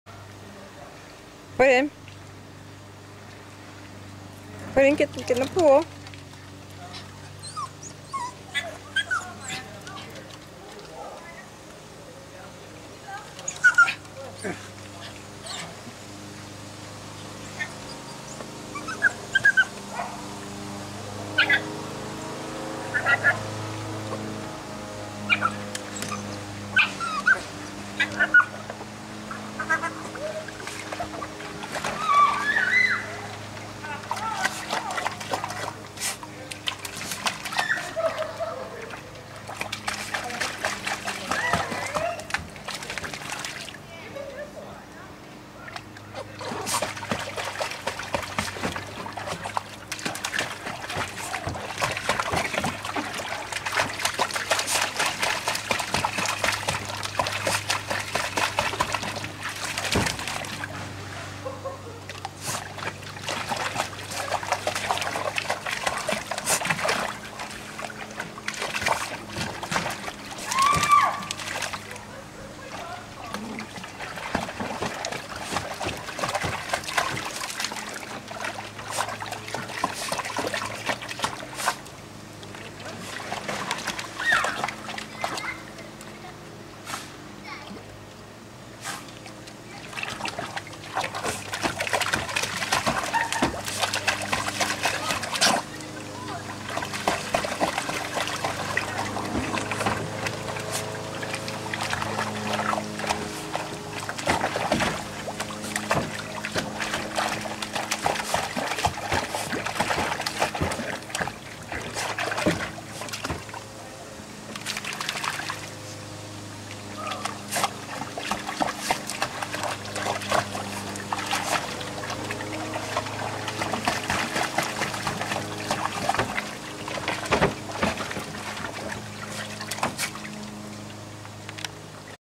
Puddin the cute Jack
Cute Jack Russell called Puddin plays in kiddy pool